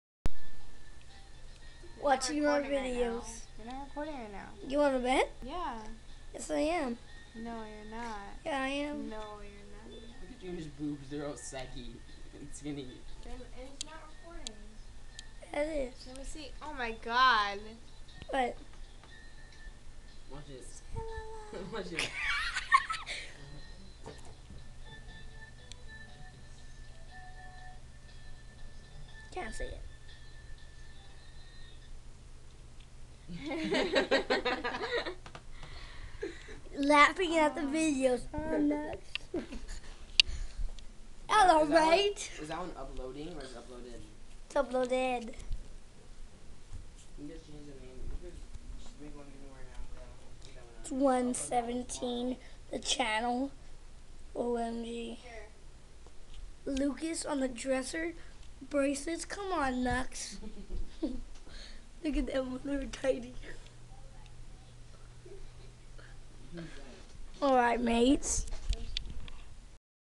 bcp: jr takes over part 1
nikki didnt know jr was filming til later, haha way to go jr, keep talking